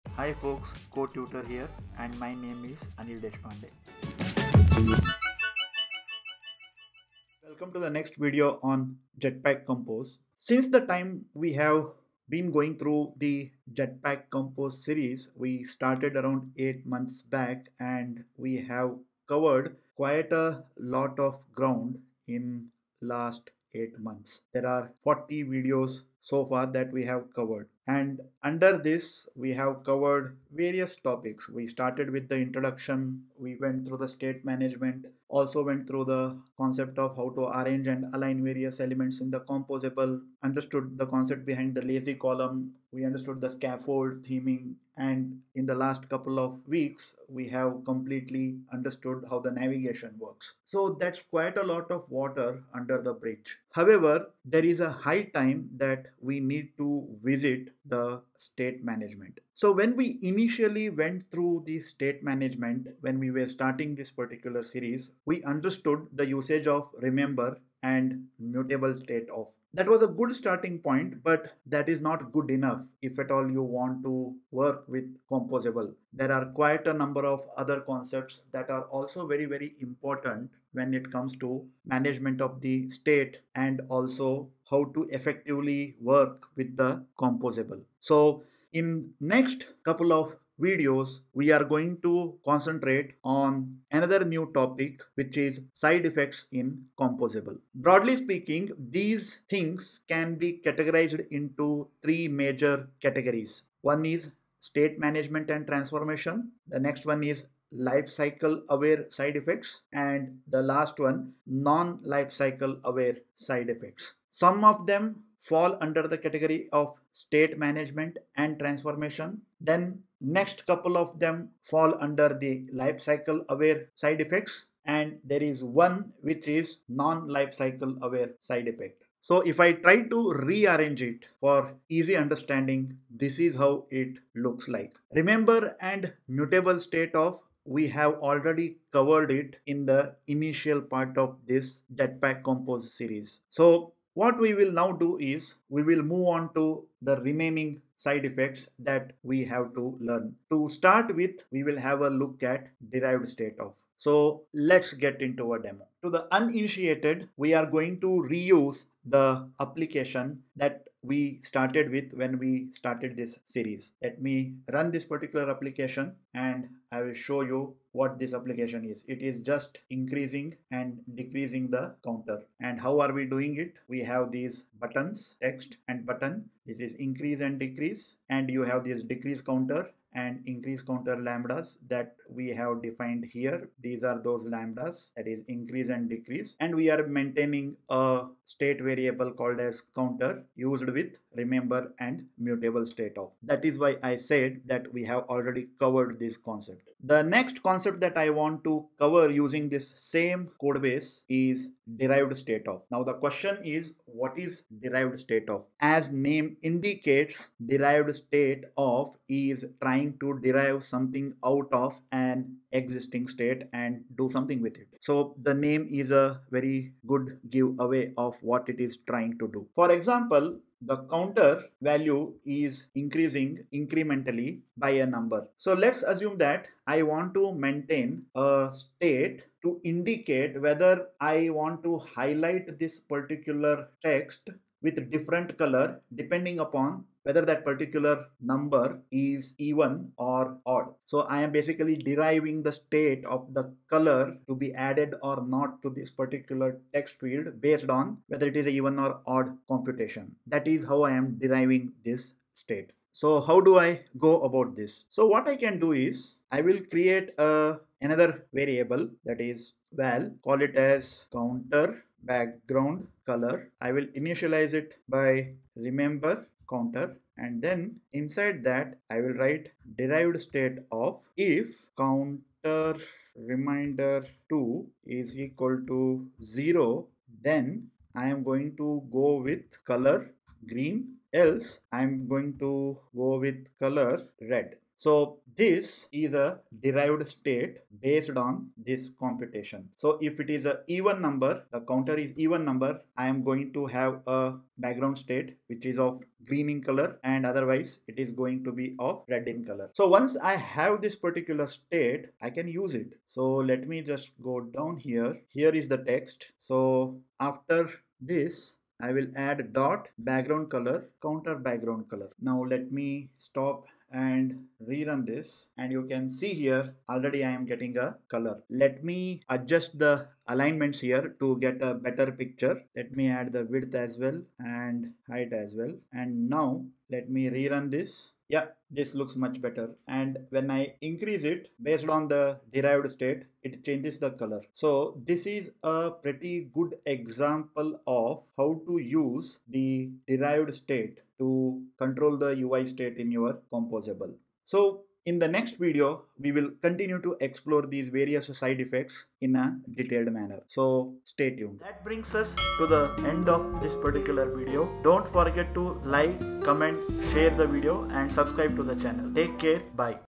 Intro to SideEffects | derivedStateOf : Android SideEffects | Jetpack Compose [Kotlin] - 41
With 40 videos under our belt, we've delved into a myriad of topics, from the basics of State Management to the intricacies of navigation within the Jetpack Compose framework. Today, we're circling back to an essential topic that deserves a deeper dive: State Management, with a special focus on side effects in composable functions.

In this tutorial, we're introducing a pivotal concept in Android development - `derivedStateOf`. This powerful tool allows developers to derive state changes from existing states, enhancing UI responsiveness and interactivity. Through a practical demo, we showcase how to use `derivedStateOf` to dynamically change the background color of a text field based on whether a number is even or odd, providing a clear, hands-on example of its utility in real-world applications.

Whether you're a seasoned developer looking to refine your skills in Jetpack Compose or a beginner eager to grasp the fundamentals of Android development, this video offers valuable insights into managing state effectively and utilizing side effects to create more dynamic and responsive apps.

Don't miss out on this opportunity to elevate your Android development skills. Your journey to mastering Jetpack Compose continues here!

Chapters
0:00 - Introduction & Series Overview
0:37 - Recap of Topics Covered
1:01 - Revisiting State Management
1:35 - New Topic: Side Effects in Composable
2:34 - Demo: `derivedStateOf`
5:45 - Conclusion & Next Steps